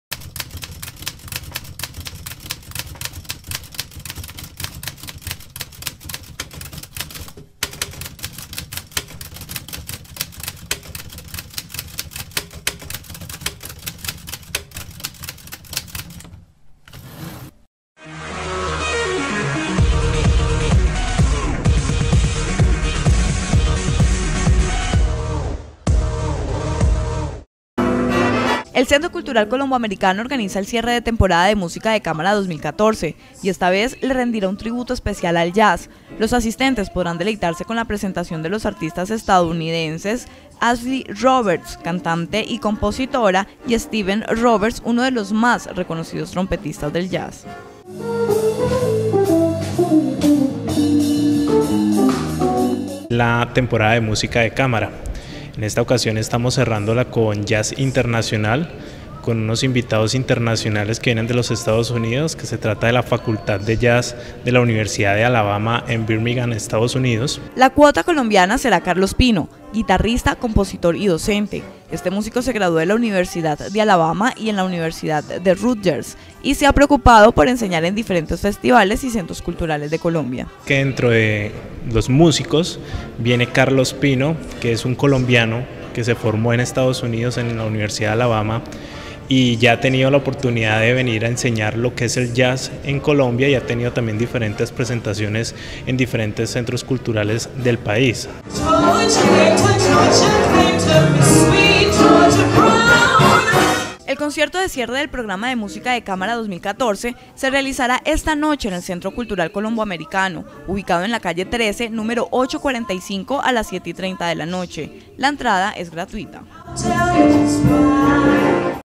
Esta noche reconocidos exponentes del Jazz darán concierto gratis
El Centro Cultural Colombo Americano organiza el cierre de temporada de música de cámara 2014 y esta vez le rendirá un tributo especial al Jazz. Los asistentes podrán deleitarse con la presentación de artistas como la cantante y la compositora estadounidense Ashley Roberts  y su paisano Steven Roberts uno de los más reconocidos trompetistas del Jazz.

La cuota colombiana será Carlos Pino, guitarrista, compositor y docente. Este músico se graduó en la Universidad de Alabama y en la Universidad de Rutgers y se ha preocupado por enseñar  en diferentes festivales y centros culturales de Colombia.

El concierto de cierre del programa de música de cámara 2014 se realizará esta noche en el Centro Cultural Colombo Americano  ubicado en la calle 13 número 6N- 40 a las 7:30 p.m. La entrada es gratuita.